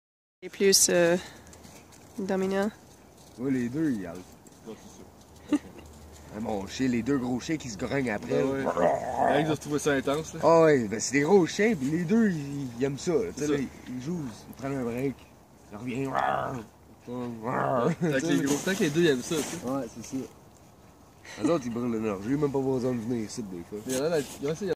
Aussie Shep vs corgi: a battle of ruv!
Duffy just can't get enough of those Aussies💜